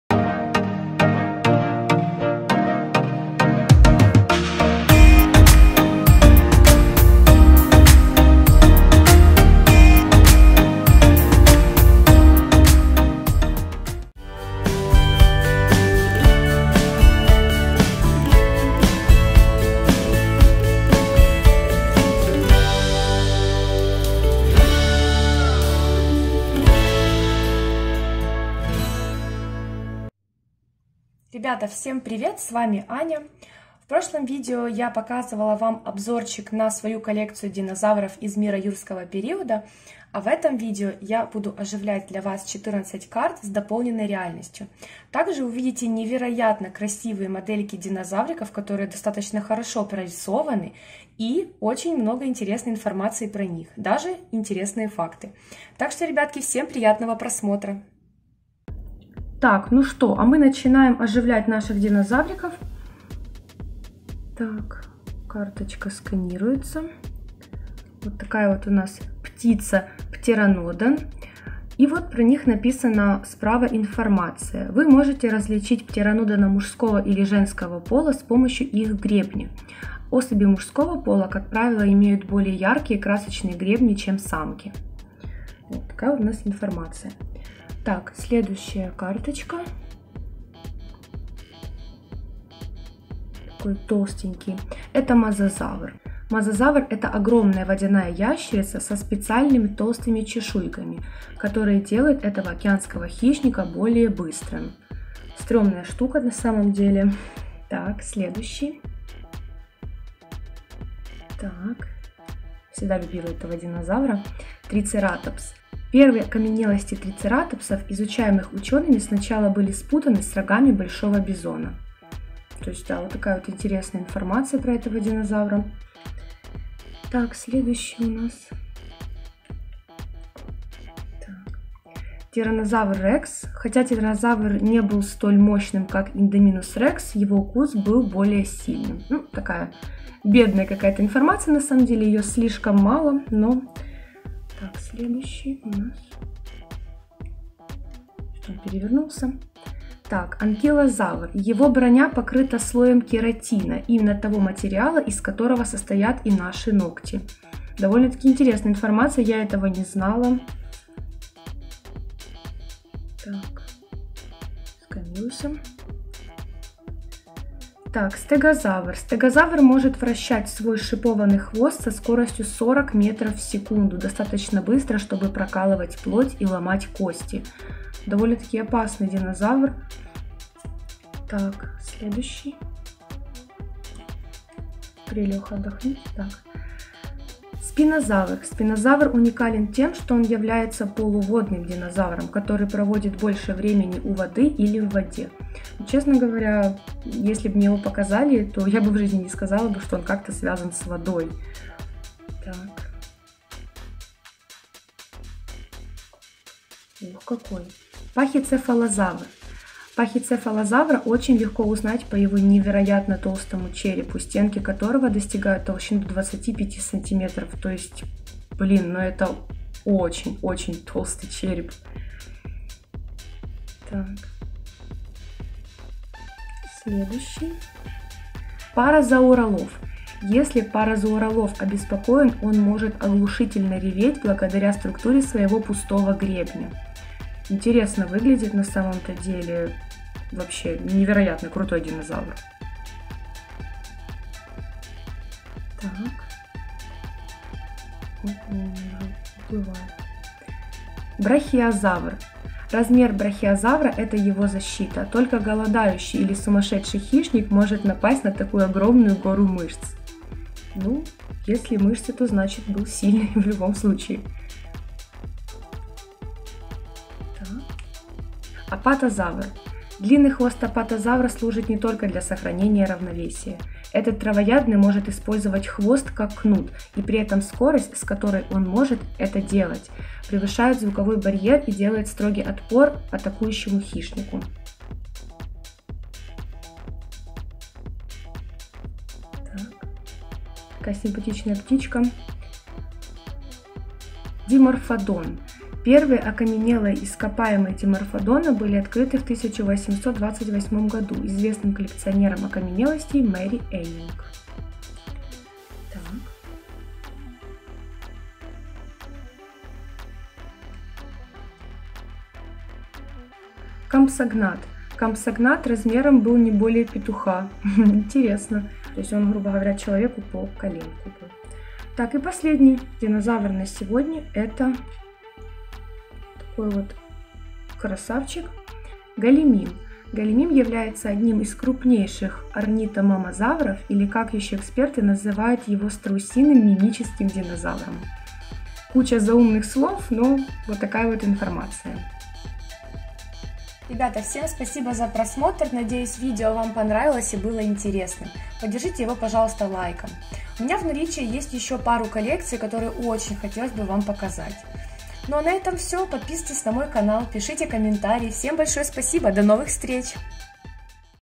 Оживляю карточки Динозавров - динозавры РЕАЛЬНЫ?!!! 3D Модели Динозавров Varus
Всем привет, как и обещала, сделала видео про карточки динозавров, где я их оживляю. Много интересной и забавной информации в этом видео.